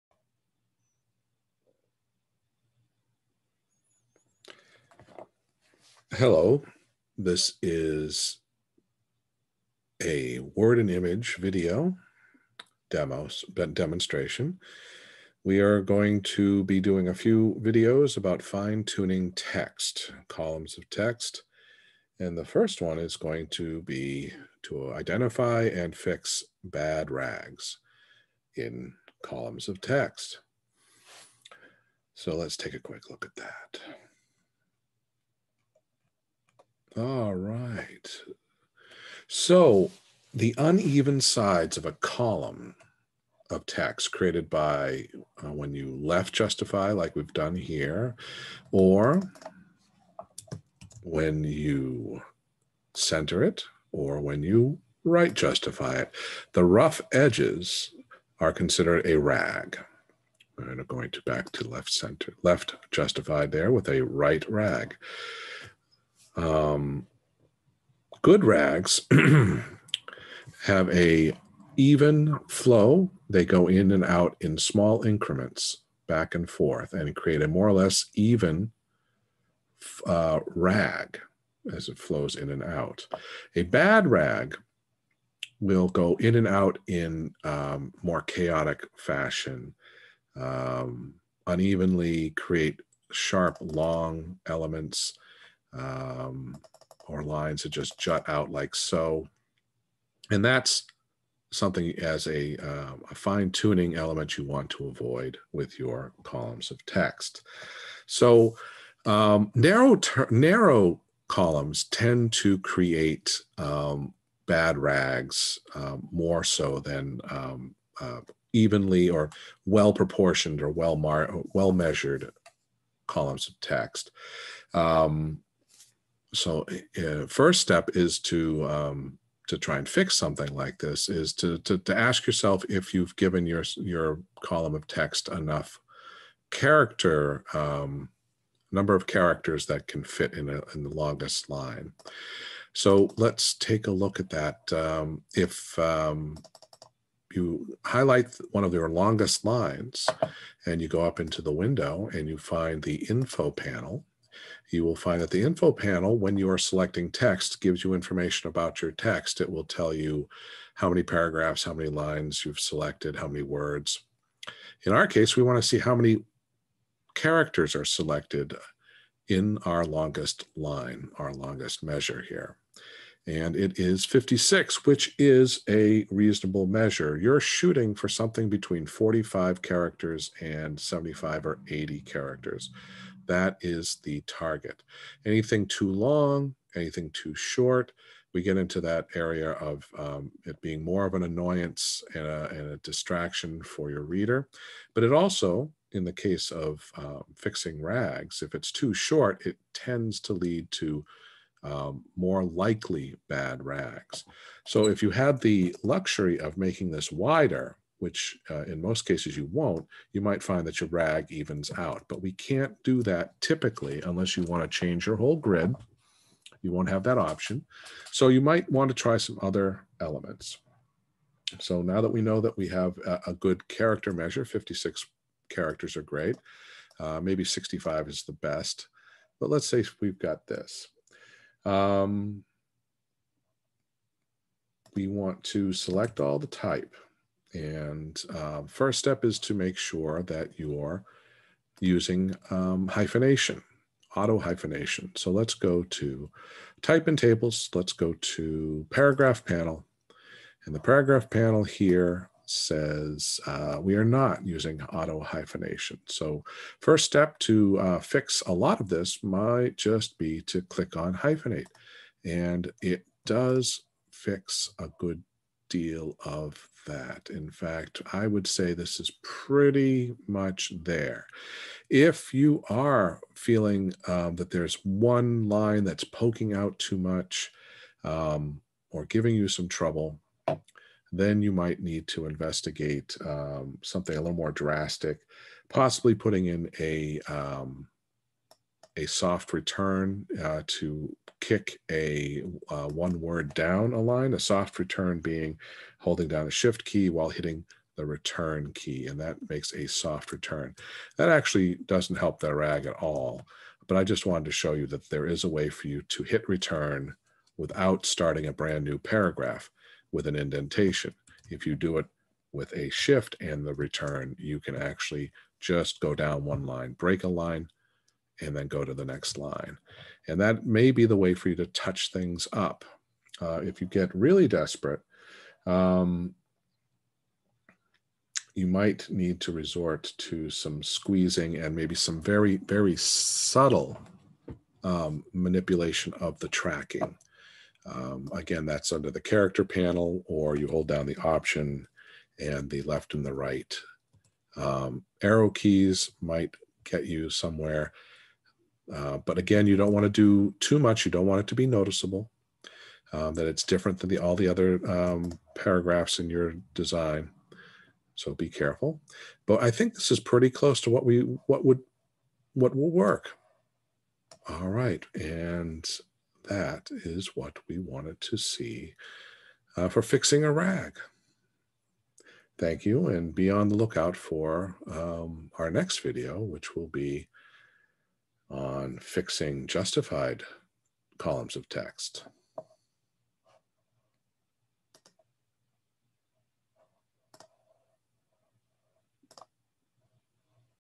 Fixing Rags in Text in Adobe InDesign
This Adobe InDesign tutorial demonstrates how to identify and fix bad rags in columns of text.